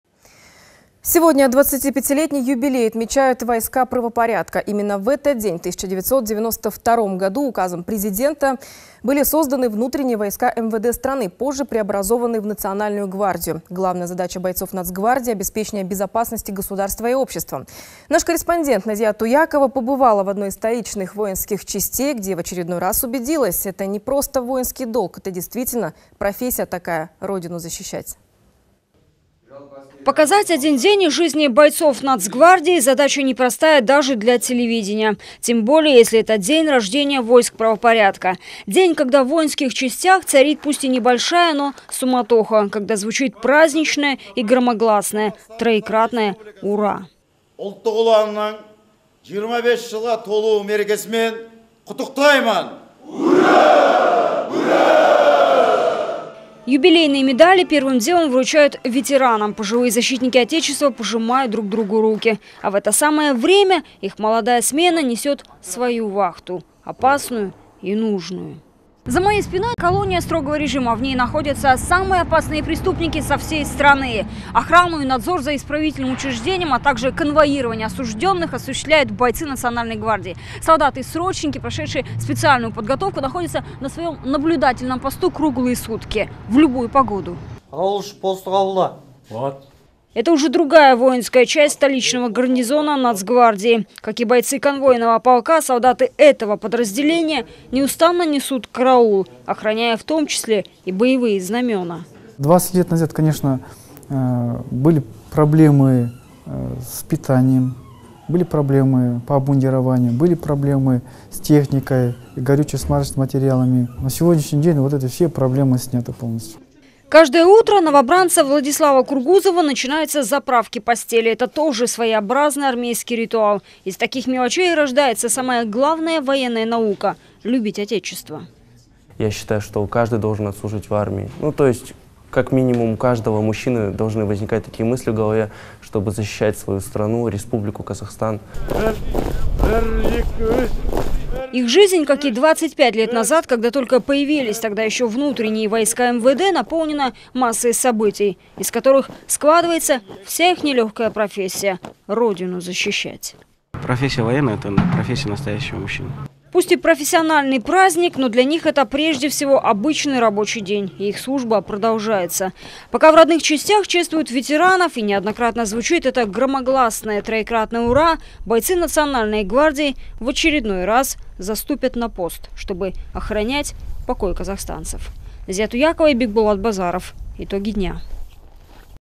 Сегодня 25 лет национальной гвардии РК
Сегодня 25-летний юбилей отмечают войска правопорядка. Именно в этот день в 1992 году указом Президента были созданы внутренние войска МВД страны, позже преобразованные в национальную гвардию.

Главная задача бойцов нацгвардии – обеспечение безопасности государства и общества. Наш корреспондент Назия Туякова побывала в одной из столичных воинских частей, где в очередной раз убедилась, это не просто воинский долг, это, действительно, профессия такая – родину защищать.

Показать один день из жизни бойцов нацгвардии – задача непростая даже для телевидения.  Тем более, если это день рождения войск правопорядка, день, когда в воинских частях царит небольшая, но суматоха, когда звучит праздничное и громогласное троекратное ура.

Юбилейные медали первым делом вручают ветеранам. Пожилые защитники отечества пожимают друг другу руки. А в это самое время их молодая смена несет свою вахту, опасную и нужную.

Назия Туякова, корреспондент

- За моей спиной находится колония строгого режима. В ней находятся самые опасные преступники со всей страны. Охрану и надзор за исправительным учреждением, а также конвоирование осужденных осуществляют бойцы национальной гвардии. Солдаты-срочники, прошедшие специальную подготовку, находятся на своем наблюдательном посту круглые сутки и в любую погоду.

Это уже другая воинская часть столичного гарнизона нацгвардии. Как и бойцы конвойного полка, солдаты этого подразделения неустанно несут караул, охраняя, в том числе и боевые знамена.